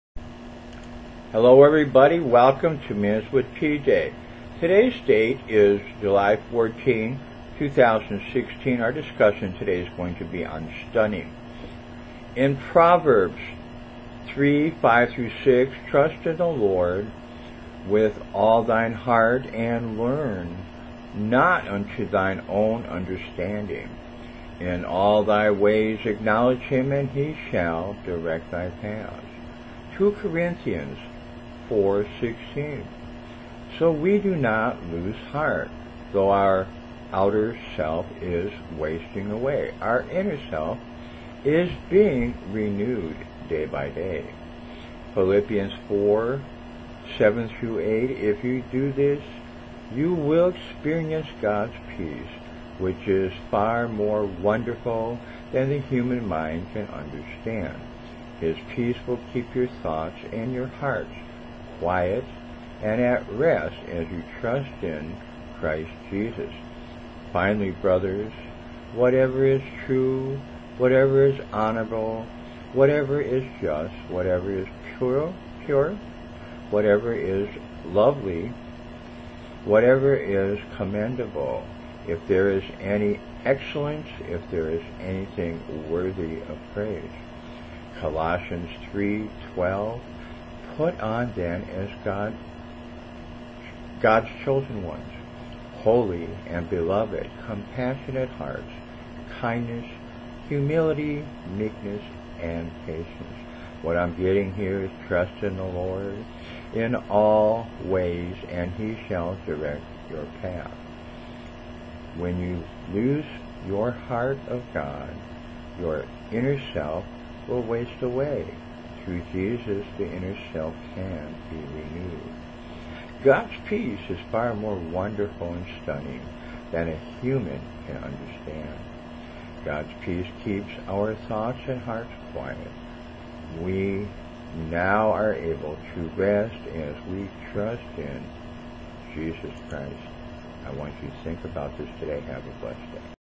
2 Minutes with PJ 7/14/2016, Stunning
(Prov 3:5-6 KJV)  Trust in the LORD with all thine heart; and lean not unto thine own understanding. In all thy ways acknowledge him, and he shall direct thy paths.
(2 Corinthians 4:16) So we do not lose heart. Though our outer self is wasting away, our inner self his being renewed day by day.
(Phil 4:7-8 TLB)  If you do this, you will experience God's peace, which is far more wonderful than the human mind can understand. His peace will keep your thoughts and your hearts quiet and at rest as you trust in Christ Jesus. Finally, brothers, whatever is true, whatever is honorable, whatever is just, whatever is pure, whatever is lovely, whatever is commendable, if there is any excellence, if there is anything worthy of praise,
Colossians 3:12 Put on then, as God’s chosen ones, holy and beloved, compassionate hearts, kindness, humility, meekness, and patience…